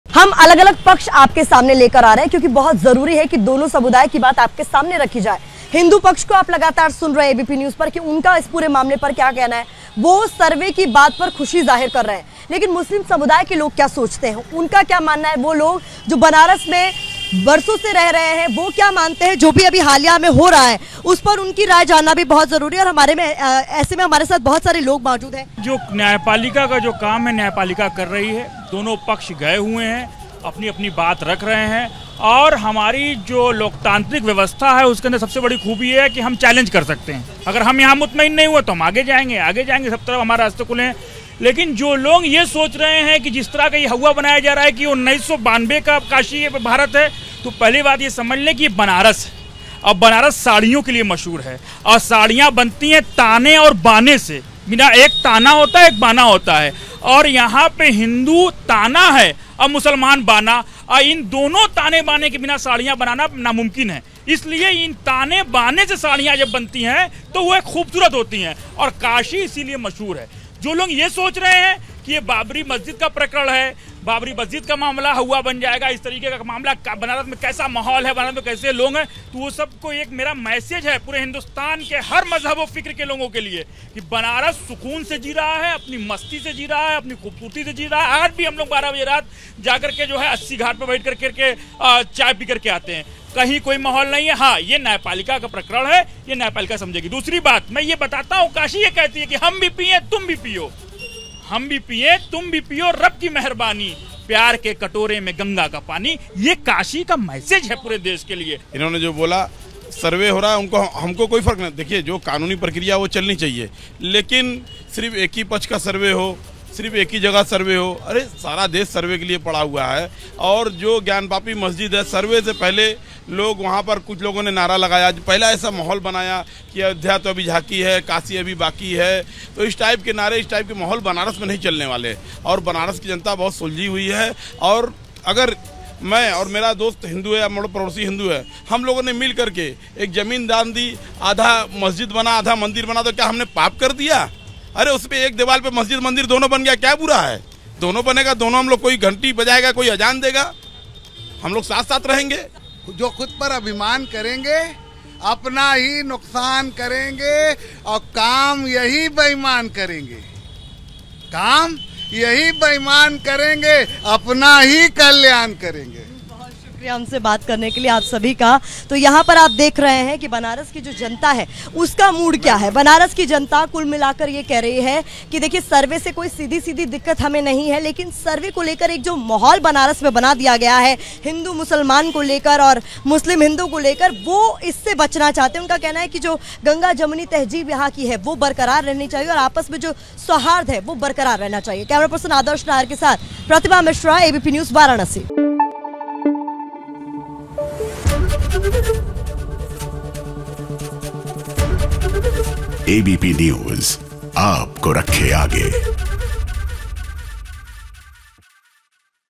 Gyanvapi Masjid Survey पर बनारस के इस मुस्लिम को जरूर सुनिए
आज सुबह 8 बजे से वाराणसी के ज्ञानवापी में फिर सर्वे का काम शुरू हो गया है. अदालत के तय समय के मुताबिक ये सर्वे दोपहर 12 बजे तक चलेगा. ये सर्वे जब तक पूरा नहीं हो जाता तबतक जारी रहेगा. इसकी रिपोर्ट 17 मई को अदालत के सामने पेश की जाएगी.